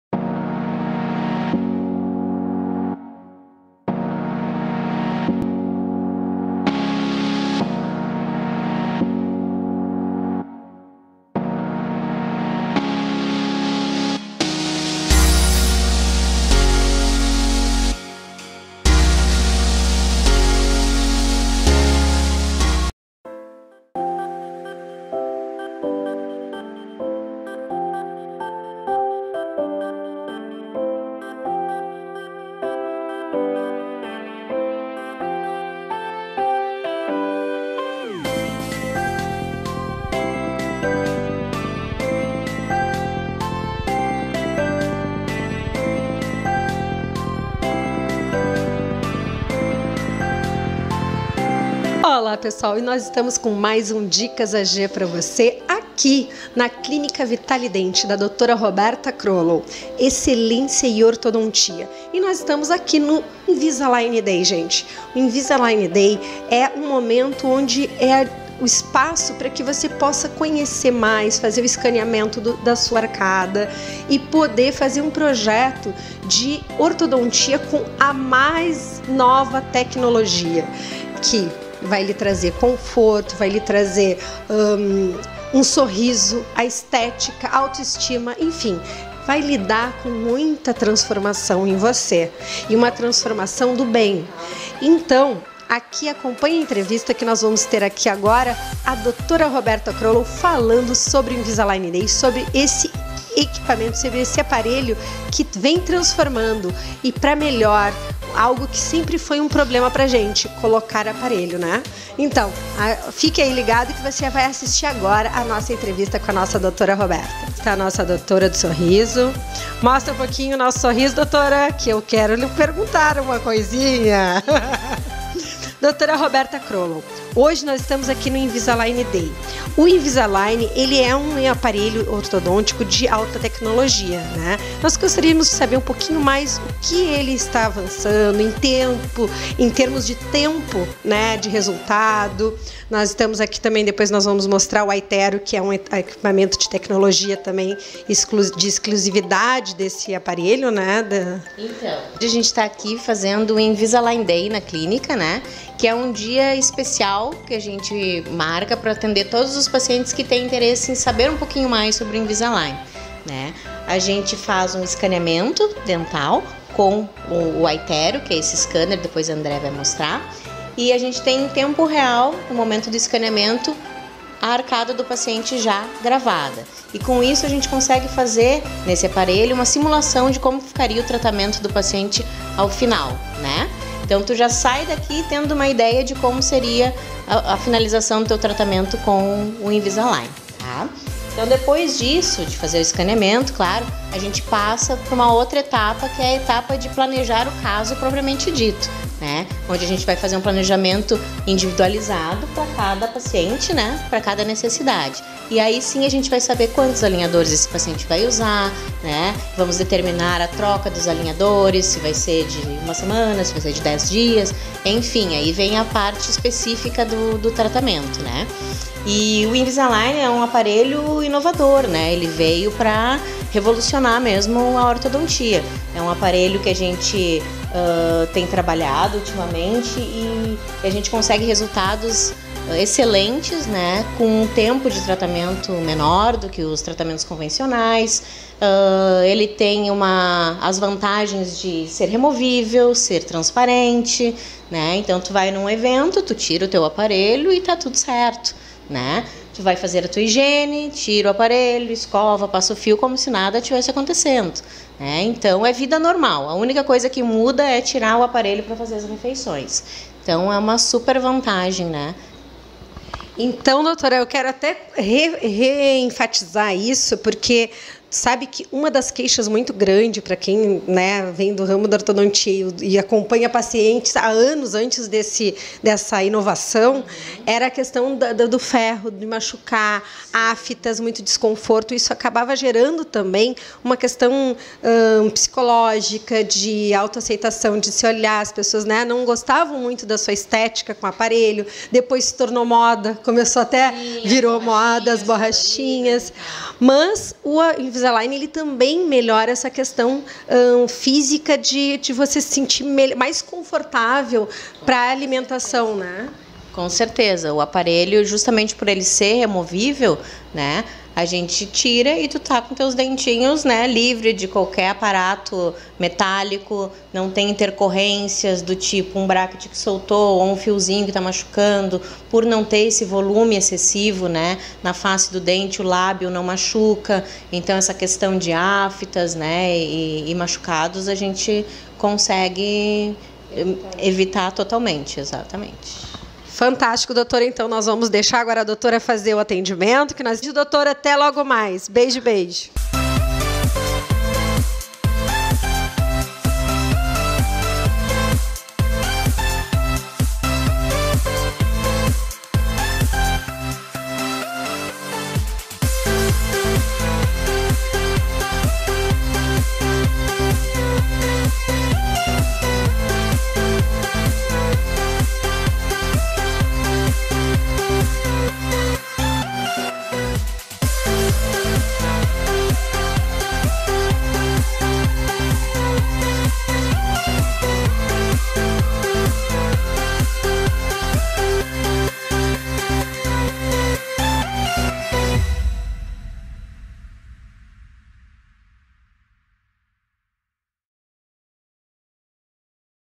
DicasAG - INVISALIGN DAY NA CLÍNICA VITALEDENTE DA DRA ROBERTA KROLOW ESPECIALISTA EM ORTODONTIA
Fomos participar do Invisalign Day na Clínica Vitaledente da Dra Roberta Krolow, especialista em Ortodontia.
Nossa Apresentadora fez a consulta e ficou motivada com a tecnologia e rapidez dos resultados, além de já ser paciente da Dra do Sorriso Roberta Krolow.
Fizeram um projeto e logo estaremos com nossa Apresentadora Andreia Grezzana utilizando o Aparelho de alta tecnologia INVISALIGN.
Ficou interessado? Agende sua avaliação com a Dra do Sorriso